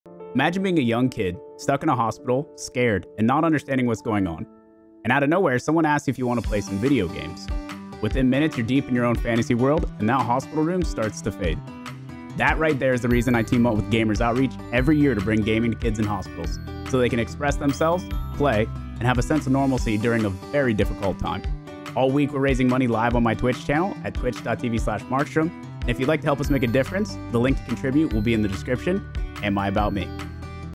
For The Kids | Gamers Outreach
MarkstromTV presents a video of markstrom announcing his participation in gamers for giving 2023!

LIVE Sunday-Friday at 0630 EST